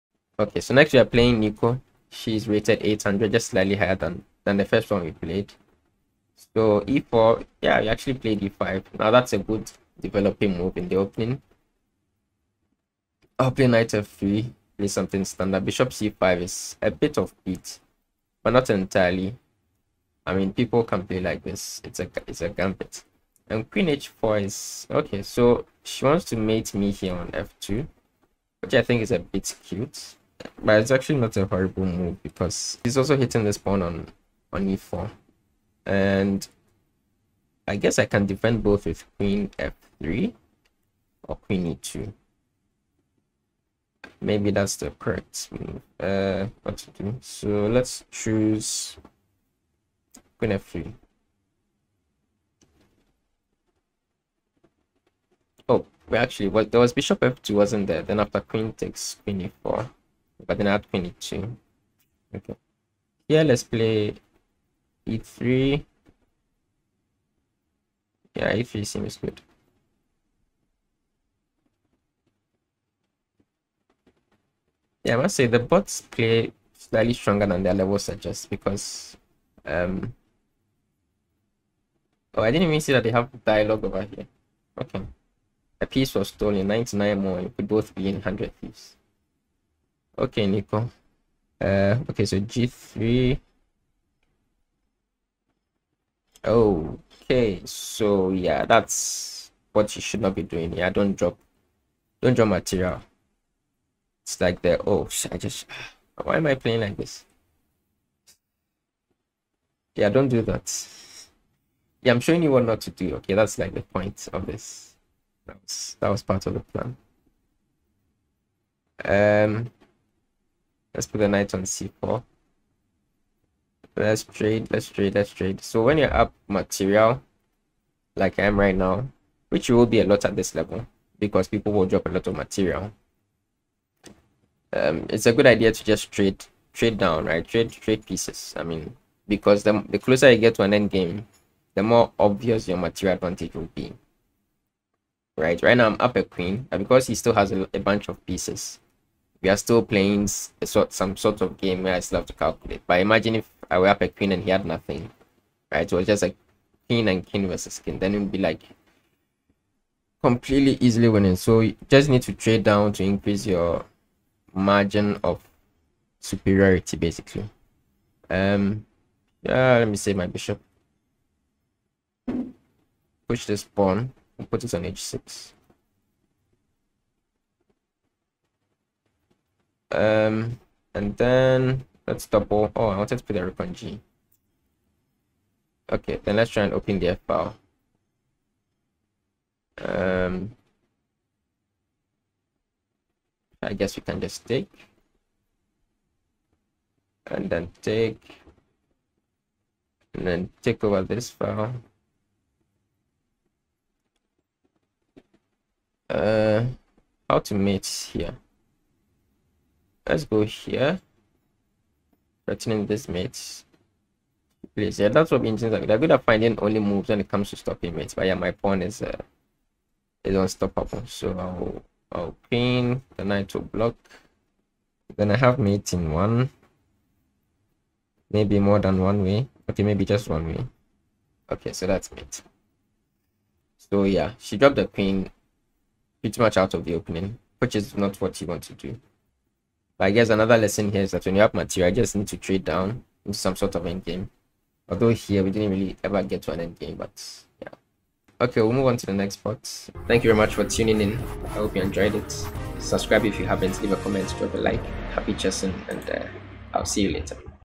DESTROYED Neeko Asks For Lessons | Vs NEEKOLUL (800) | Bot Battles #2
Welcome to Bot Battles! In these videos, we will take on some chess.com bots and point out instructive mistakes people make at those rating levels. Also look out for some not-so-funny banter with the bots.
In this video, we play against Neekolul, rated 800 ELO.
Socials:
Happy chessing!